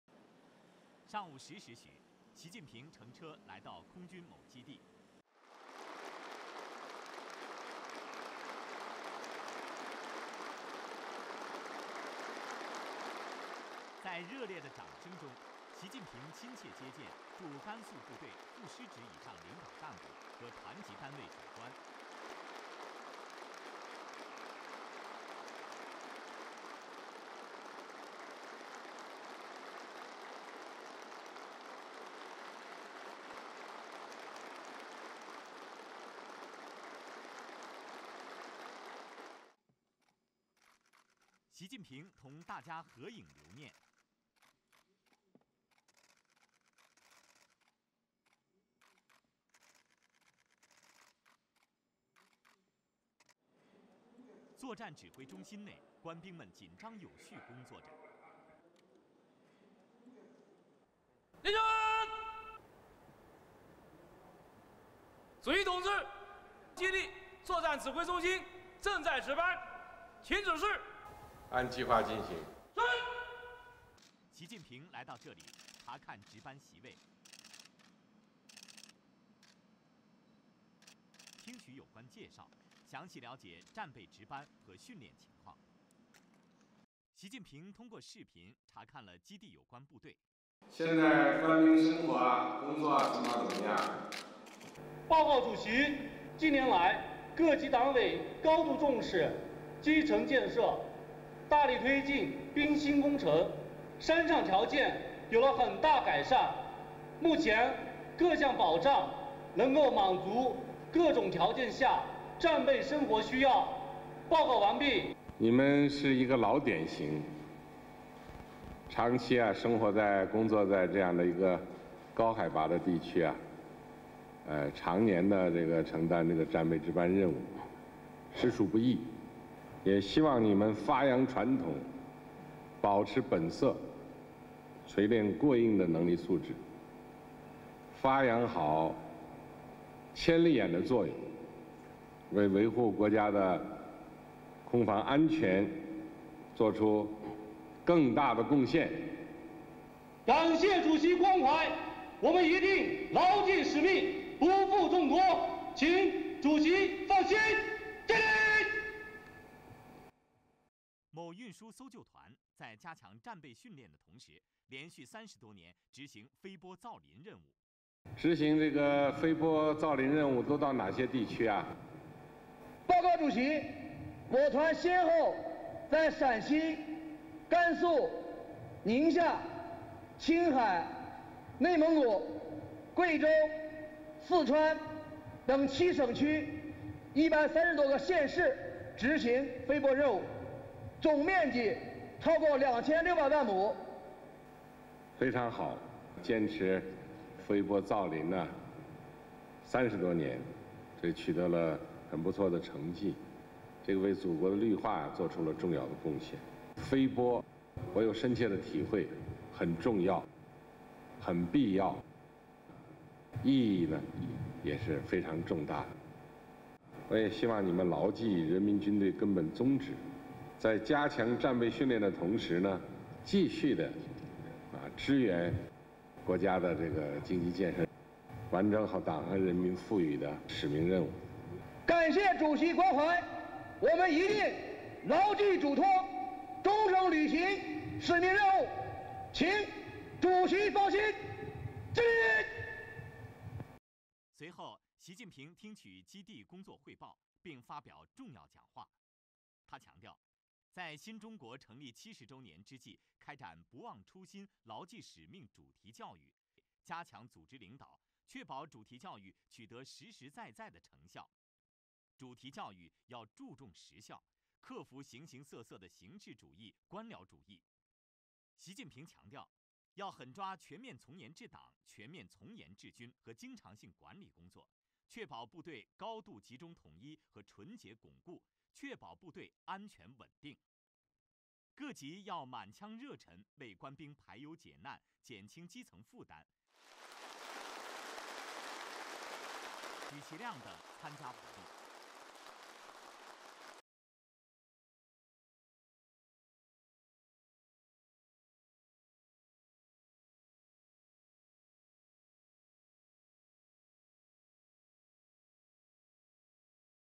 Xi Inspects PLA Air Force Base in Northwest China
Chinese President Xi Jinping inspected a base of the People's Liberation Army (PLA) Air Force in northwest China's Gansu Province on Thursday, calling on the officers and soldiers to improve their fighting capacities to greet the 70th anniversary of the founding of the People's Republic of China.